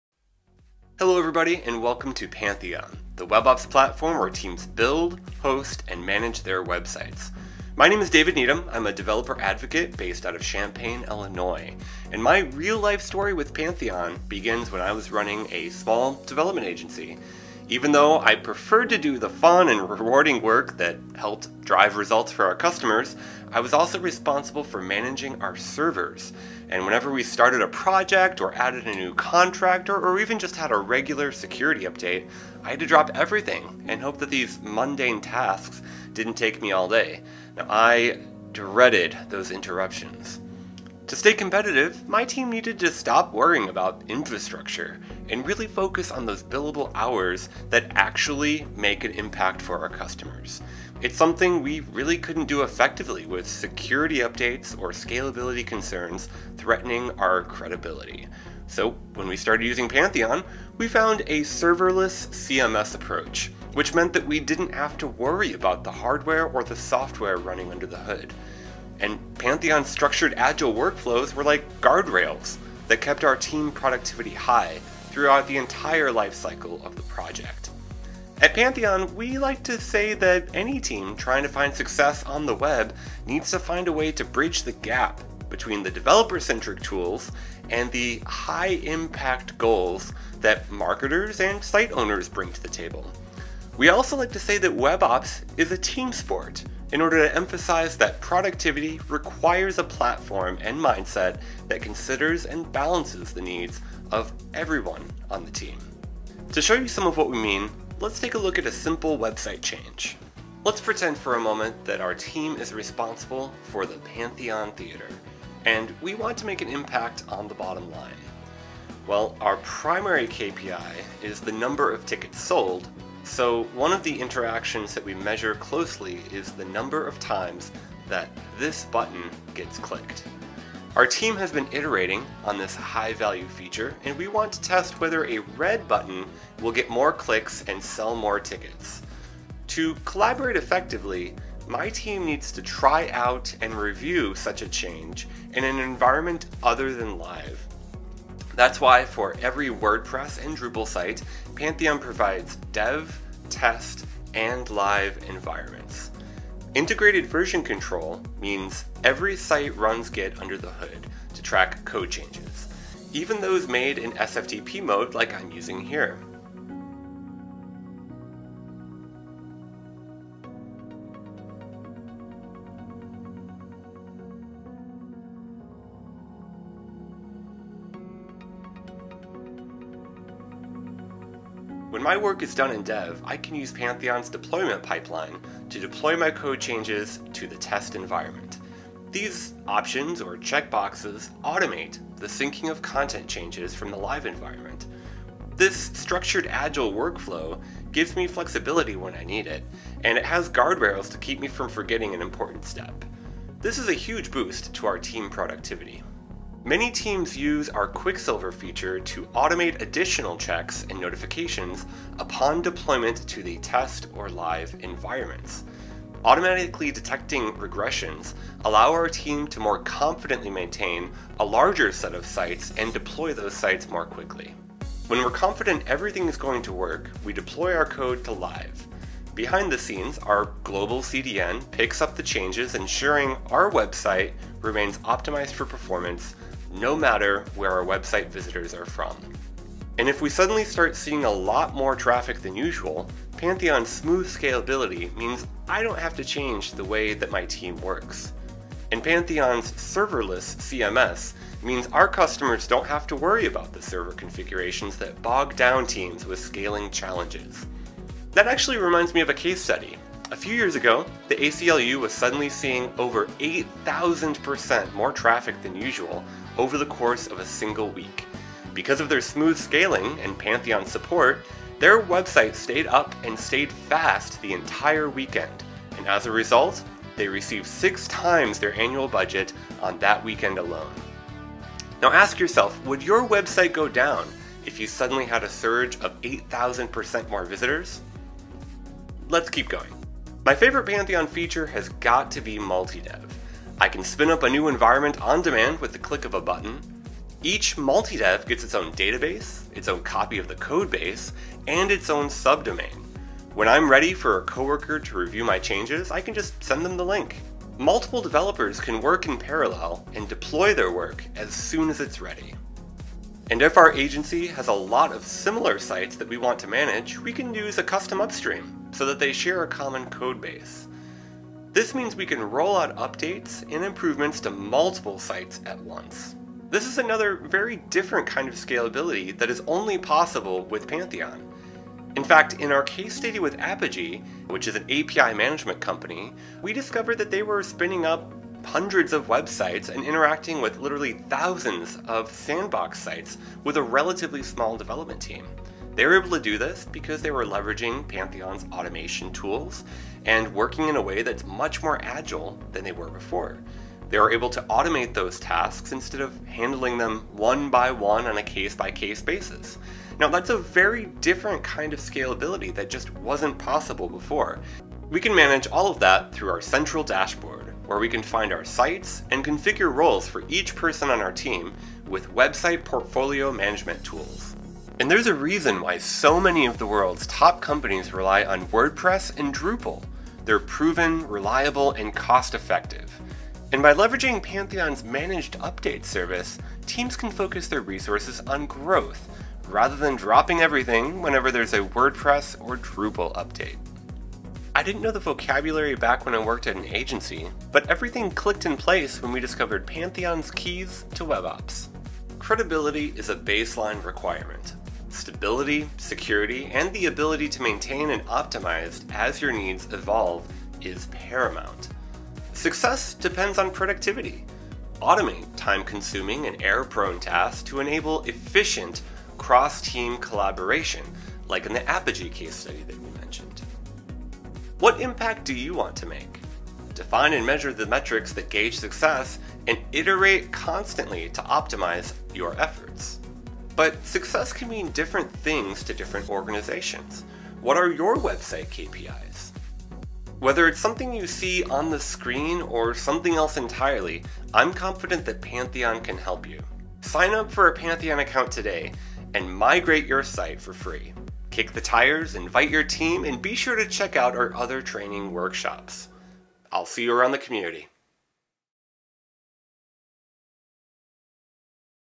Why We Moved to Pantheon – an 8-minute crash course of Pantheon's value through real-world customers
In this video David Needham, a Developer Advocate at Pantheon, shares a story from when he ran an online marketing agency. We'll hear how Pantheon's WebOps Keys allowed his team to shift wasted time and energy away from tedious maintenance towards a focus on productivity and making an impact on the bottom line for their customers.

0:00 - David Needham's Case Study
1:46 - Pantheon's Workflow
3:01 - Structured Agile Workflows
4:17 - ACLU Case Study
4:49 - Pantheon's Multidev
5:31 - APIGEE Case Study
6:14 - Managed Updates for Drupal & WordPress
6:34 - Pantheon's WebOps Keys